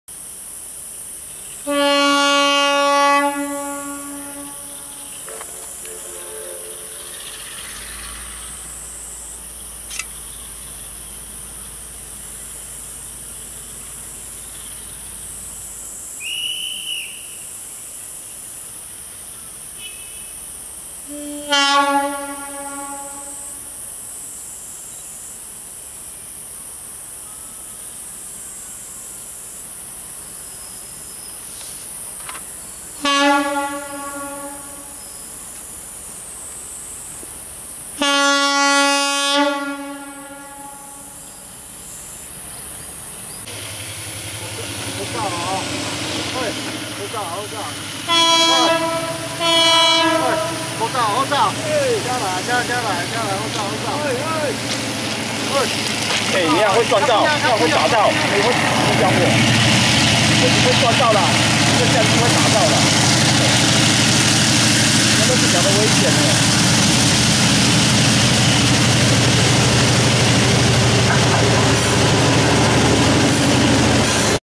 3914次勝興站、連續隧道危險動作
勝興站吹哨子、鳴笛、揮手、再鳴笛. 車到前面才閃，連續隧道又是攝影團體(歐巴桑)在鐵道中間搶鏡頭，列車已在隧道人從鐵軌中移到鐵軌旁繼續搶拍鏡頭，真是替他們緊張，為了卡位搶鏡頭只會讓駕駛壓力也影響我情緒(說話會撞到相機馬上就撞到)